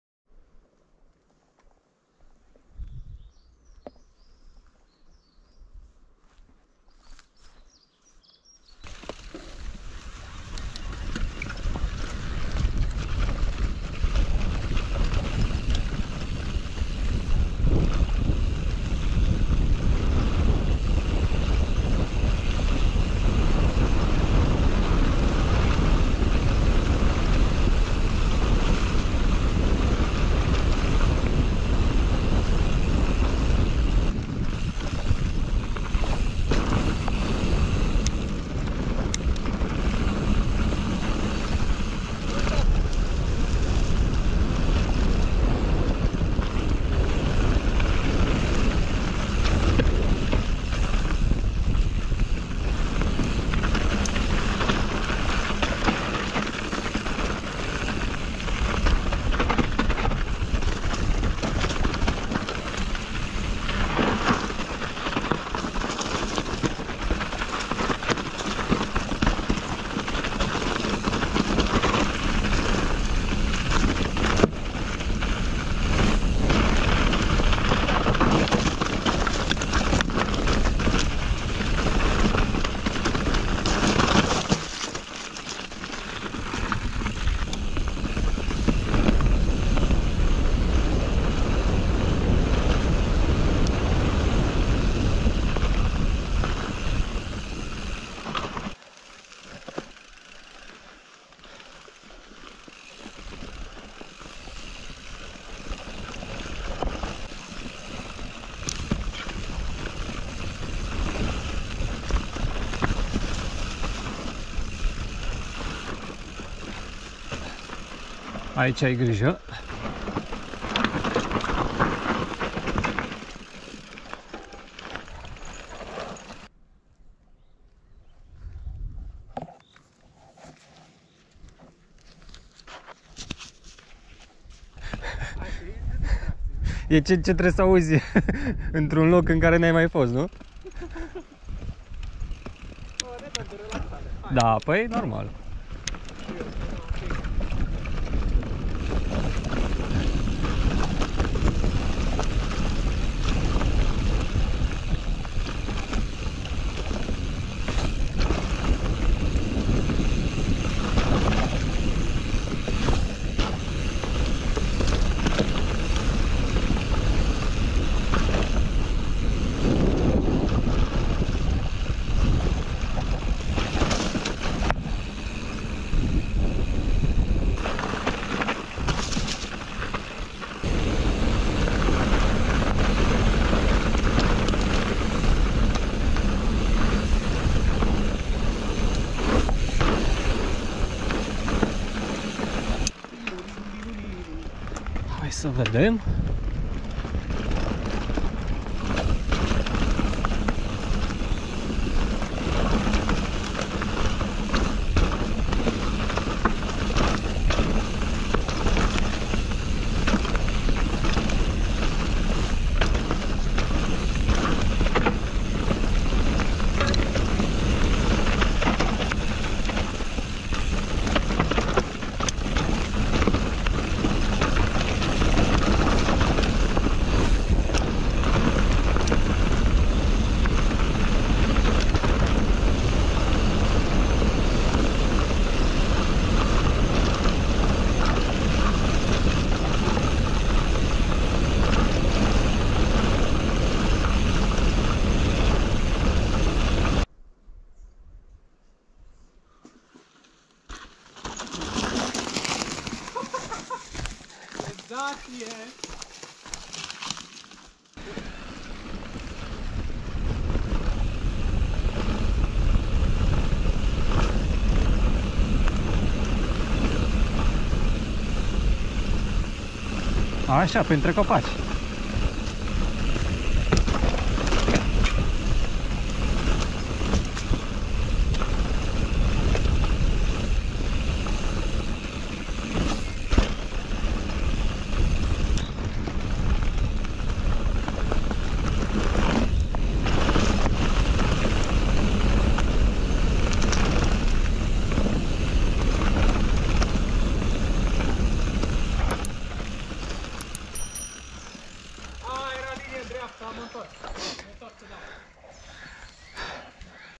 Șaua Brădetului | SĂCELE BAI PARC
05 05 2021
Coborâre din Șaua Brădetului și alte zone de pe lângă vârful Highiș - Poiana Angelescu - Săcele.

Arată-le prietenilor clipul asta

CE FOLOSESC?
Camera:  GoPro Hero 8
Edit: Davinci Resolve
Bike: Nukeproof MEGA 2021